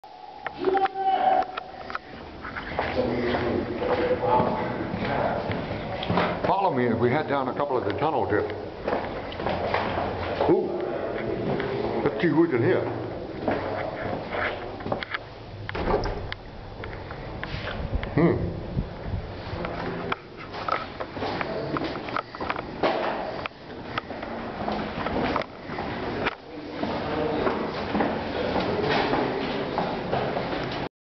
Another video from inside the tunnels
Inside the Great Siege Tunnels of Gibraltar